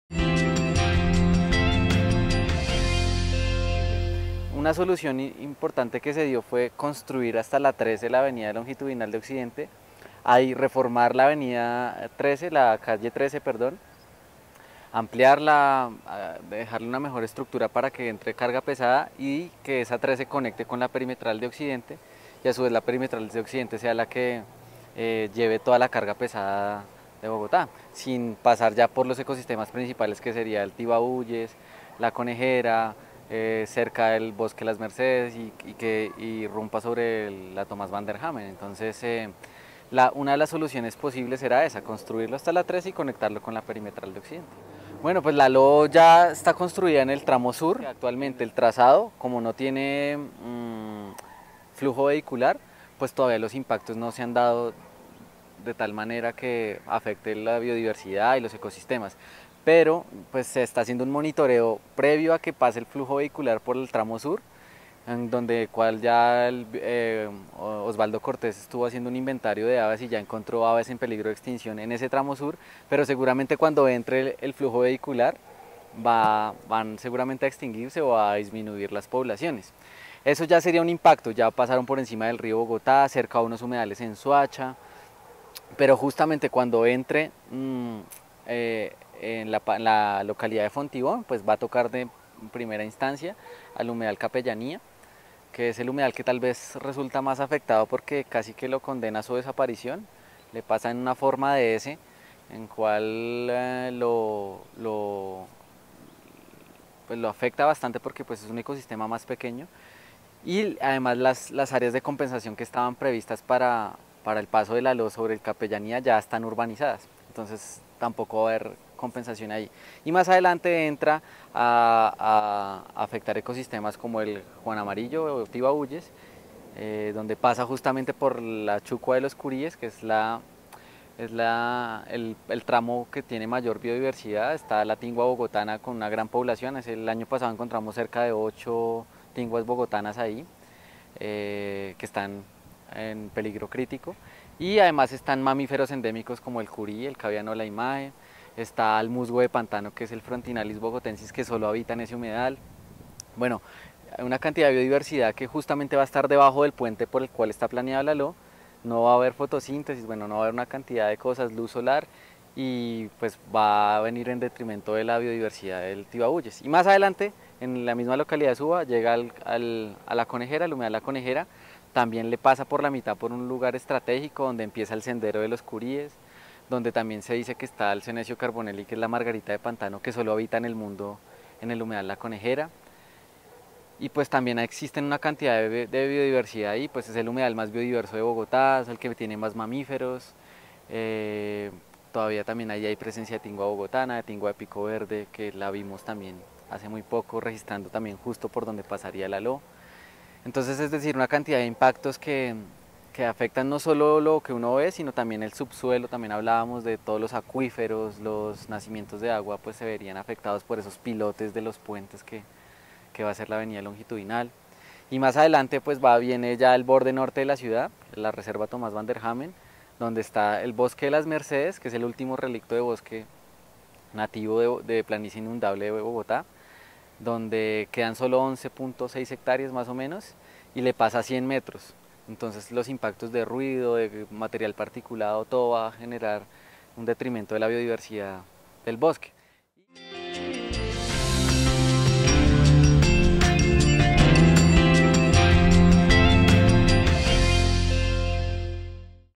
La ALO vs los humedales en Bogotá
Jorge Escobar de Humedales Bogotá nos cuenta las opciones que hay para poder hacer la ALO en Bogotá sin destruir los humedales.